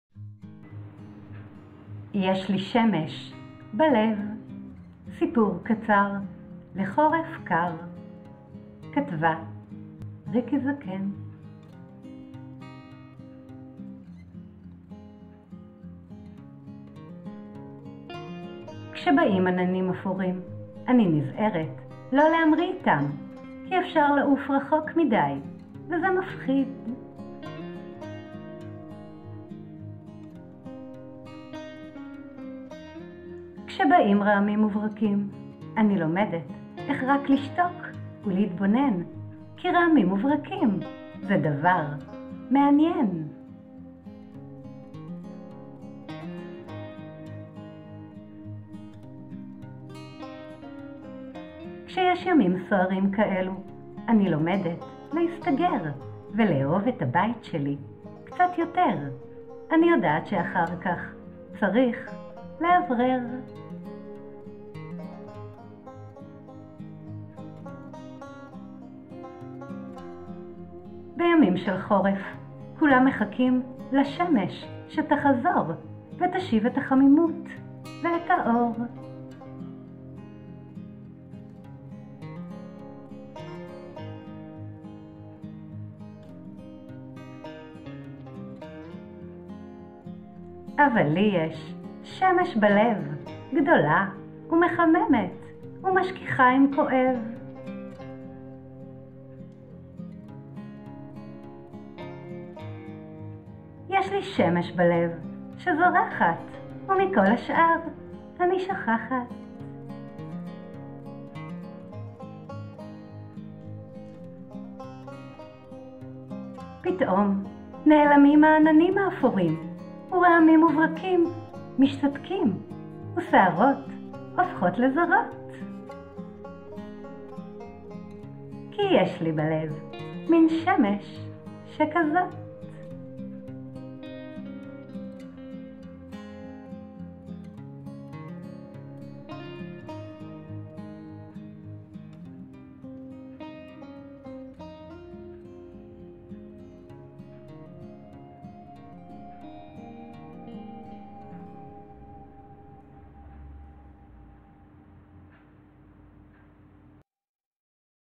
יש לי שמש בלב - סיפור קצר לחורף קר
כותבת יוצרת: ריקי זקן
יֵשׁ לִי שֶׁמֶשׁ בַּלֵּב
שֶׁזּוֹרַחַת וּמִכָּל הַשְּׁאָר
אֲנִי שׁוֹכַחַת